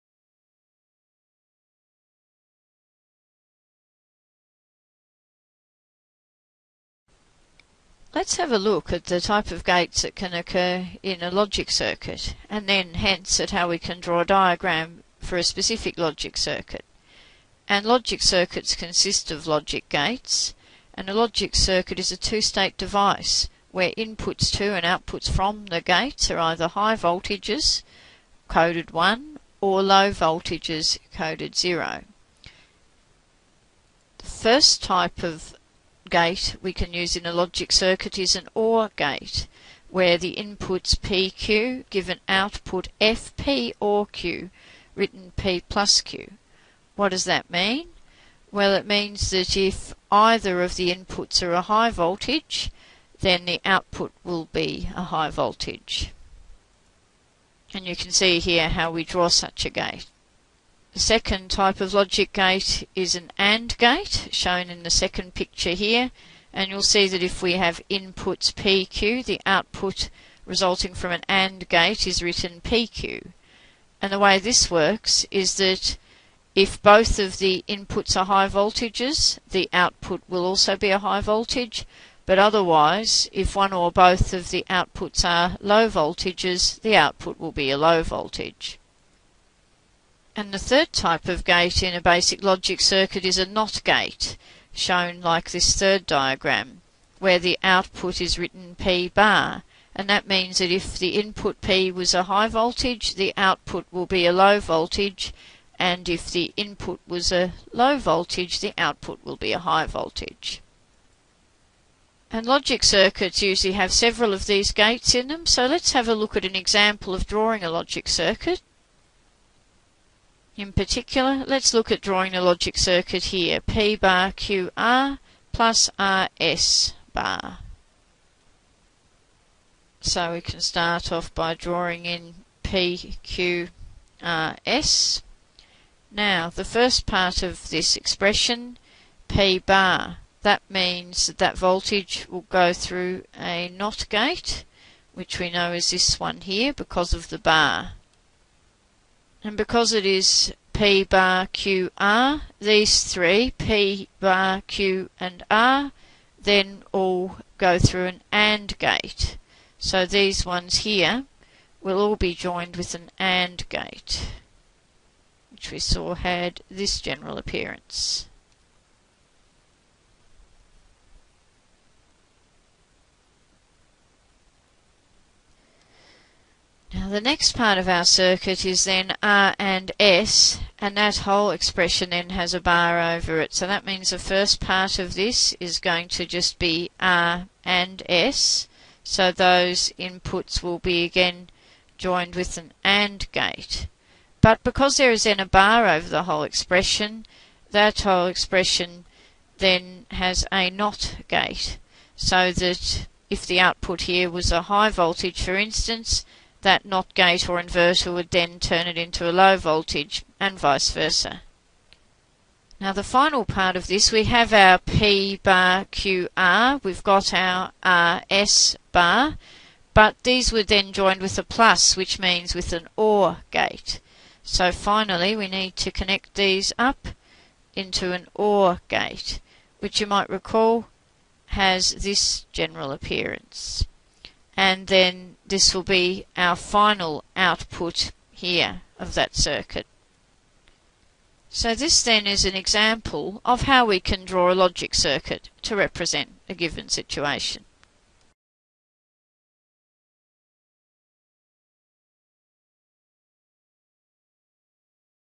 Logic circuits (MathsCasts)
This screencast begins with explanation of 'OR', 'AND' and 'NOT' gates in a logic circuit, including what each gate looks like. An example is then given of drawing a logic circuit which contains several gates.